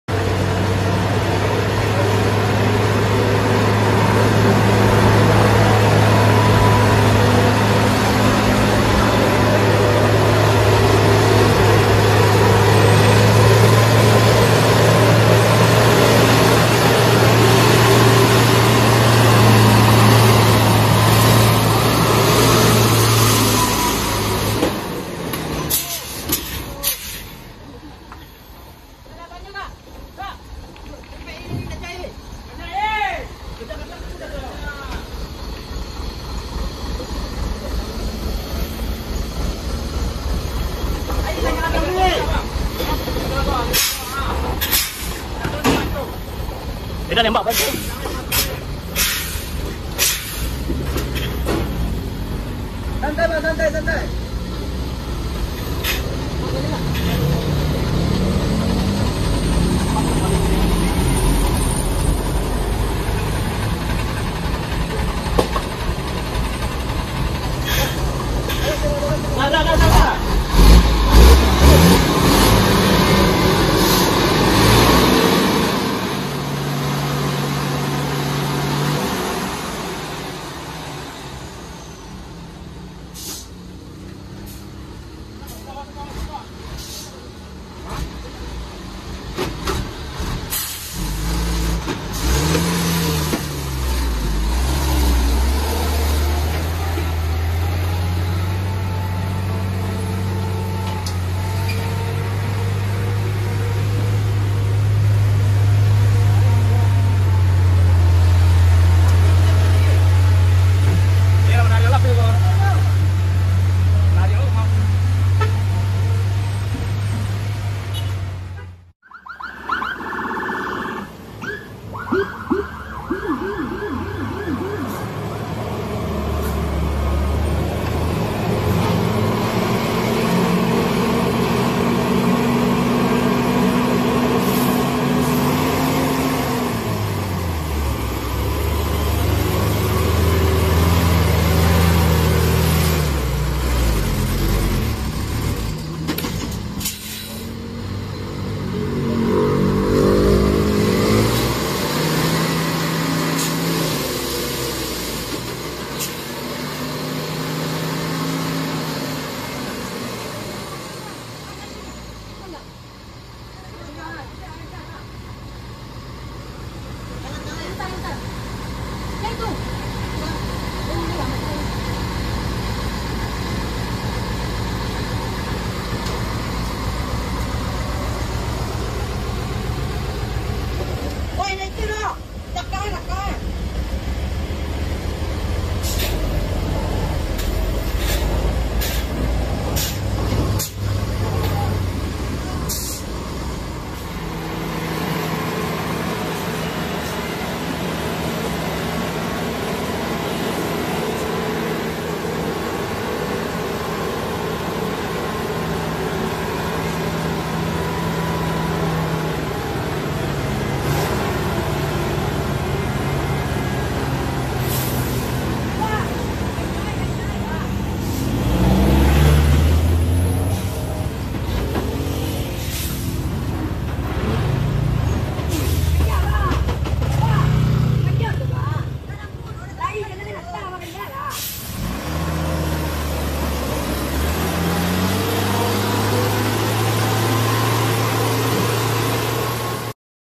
Truck Fails to Climb Hill - GIANT TRUCK !!
The Giant Truck Fails to Climb Hill
THIS TRUCK CAN'T RUN BECAUSE THE INLINE IS TOO STEEP

the red giant truck, giant truck, driver, truck drive, fuso truck, benz atego
mb actros, mercedes actros